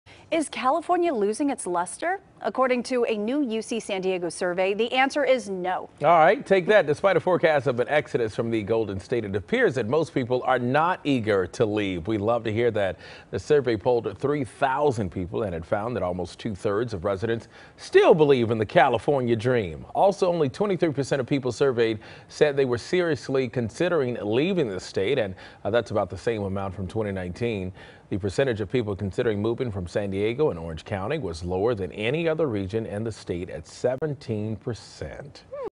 UC Study Finds No Evidence Of Touted 'Cal Exodus'
The UC San Diego survey found that just 23% of the 3,000 people polled considered leaving the state -- the same number as in 2019. DeMarco Morgan and Suzanne Marques report.